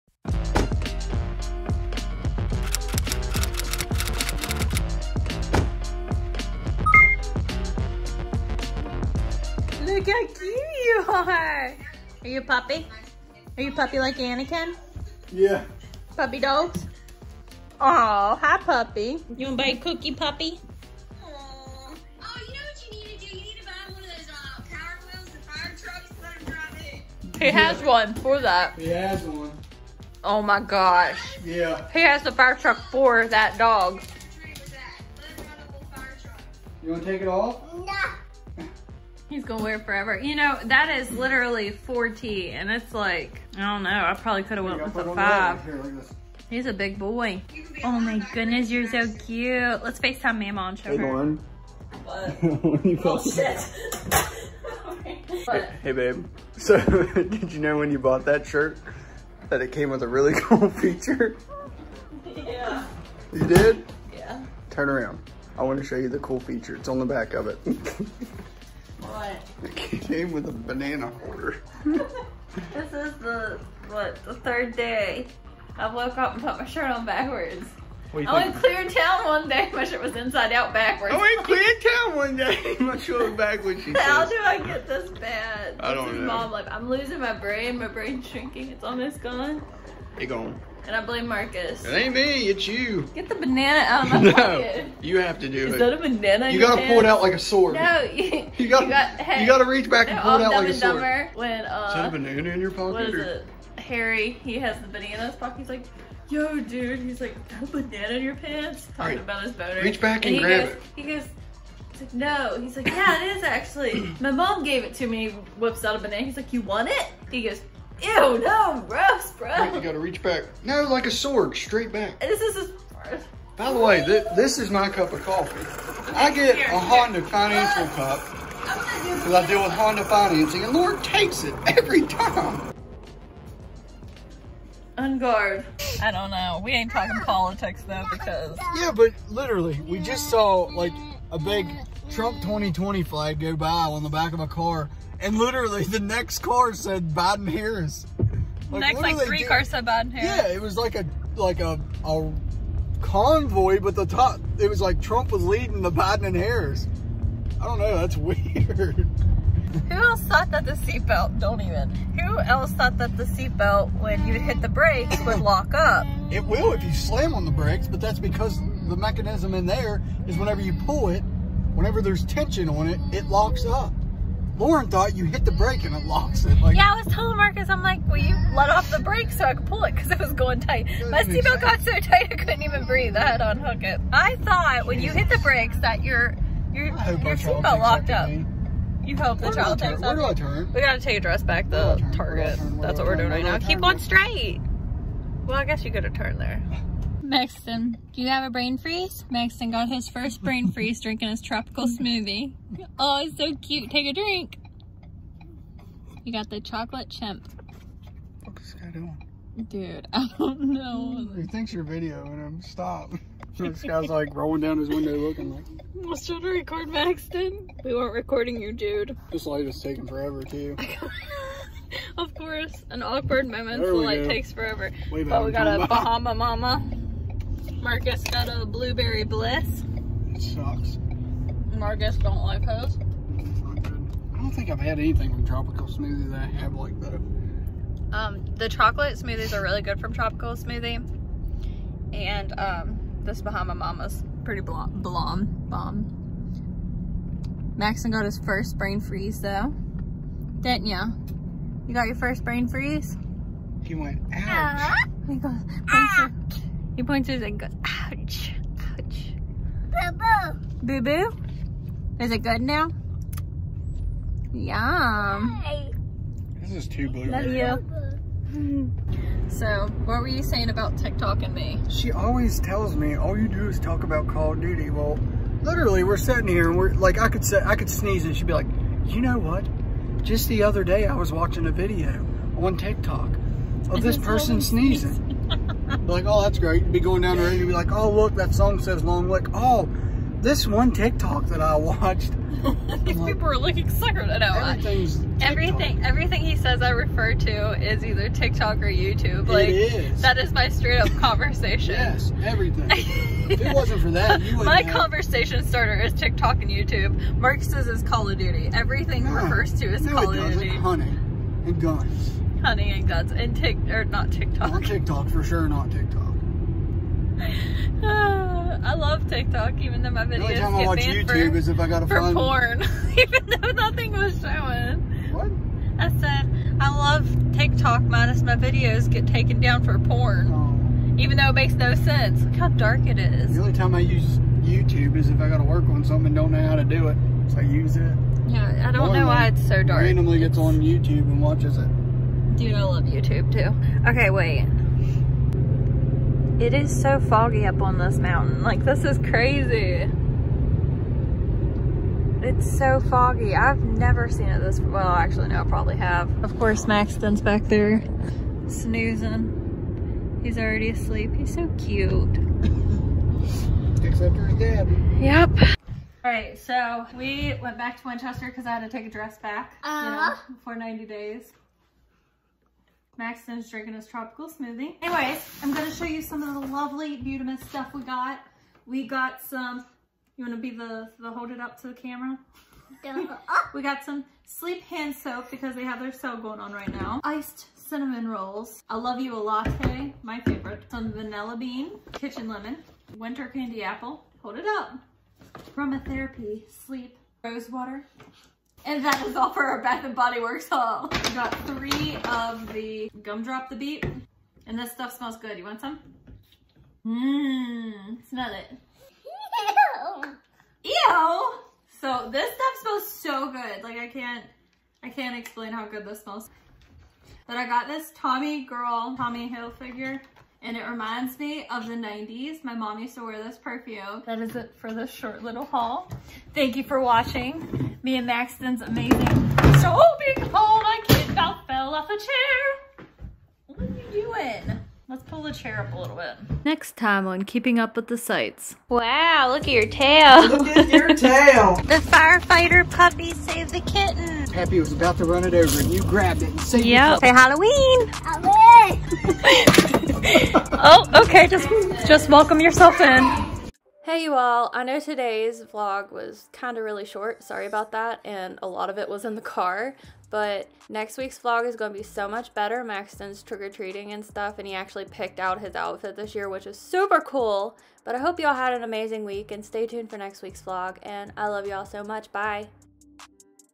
Maxton got his first brain freeze | vlog
I know this weeks vlog isn't that long or interesting. Hope you're having an amazing day and keeping a smile on your face.

Marc's Tiktok: marcussites40
Marc's Instagram: marcussites40

I love you Maxton! ♥️